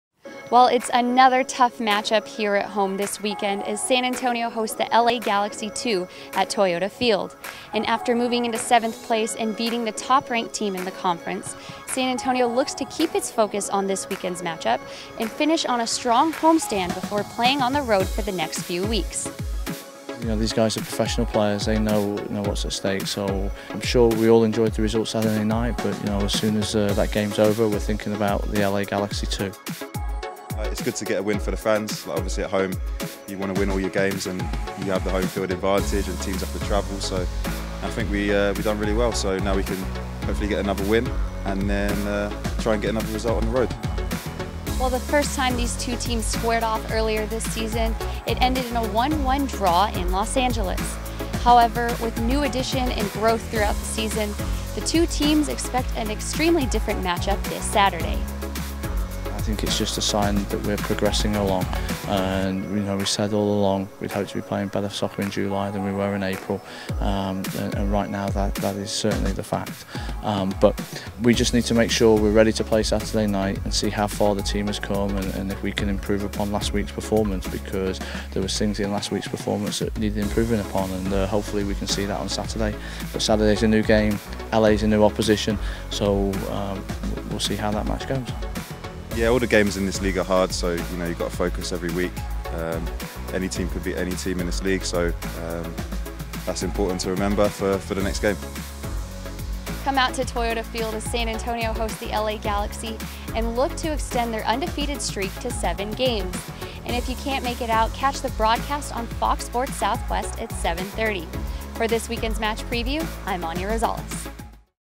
Match Preview: vs. LA Galaxy II 7/23
We preview the tilt with first place LA Galaxy II as SAFC looks to extend their unbeaten streak to 7.